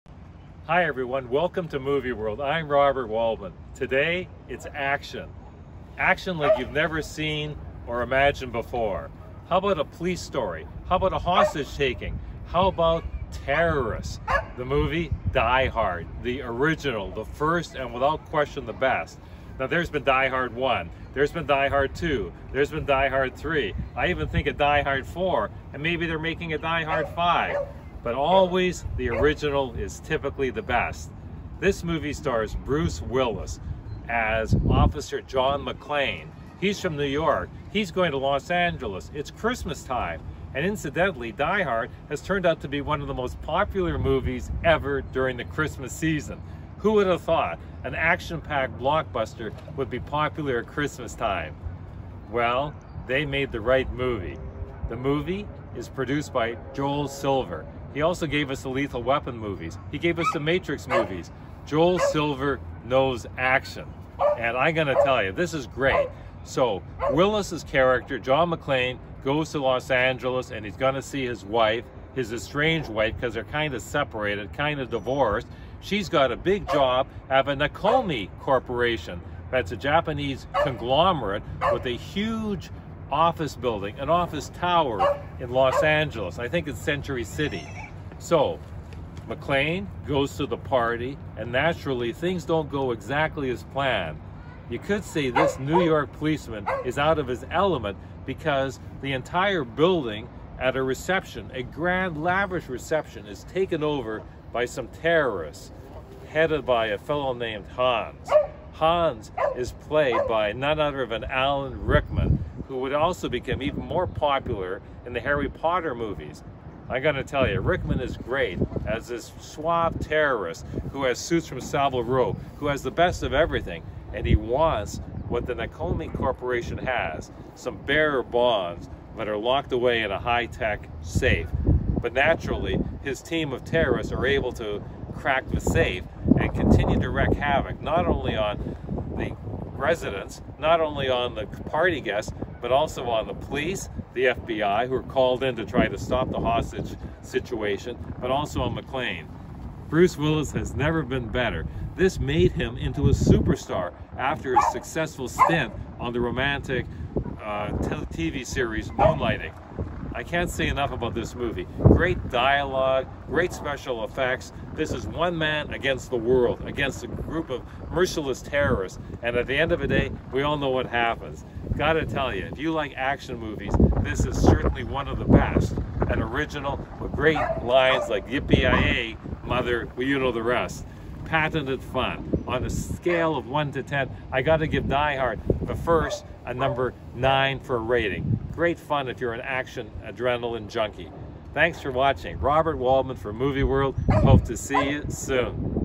Die Hard movie review by Robert Waldman The Movie World
Explosive fun as Brice Willis takes on international Terrorists holding an office tower hostage in the action-packed original Die Hard. Wall to wall fun as Brucey boy tangles with slick and sick Hans Gruber played by Harry Potter’s Alan Rickman in a bravado performance. An adrenalin high all the way, For more reviews go Thank you,